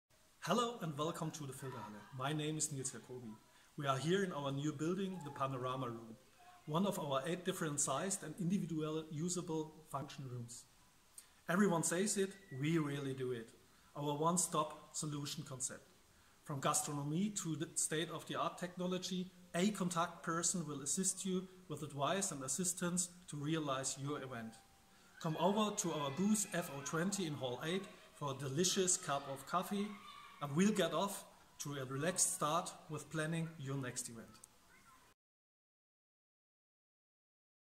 Meet the Filderhalle at IMEX Frankfurt
Exciting news: the conference center Filderhalle has a brand new extension ready to unlock ideas. Learn all about it at IMEX Frankfurt at booth F020 from 21 to 23 May 2019!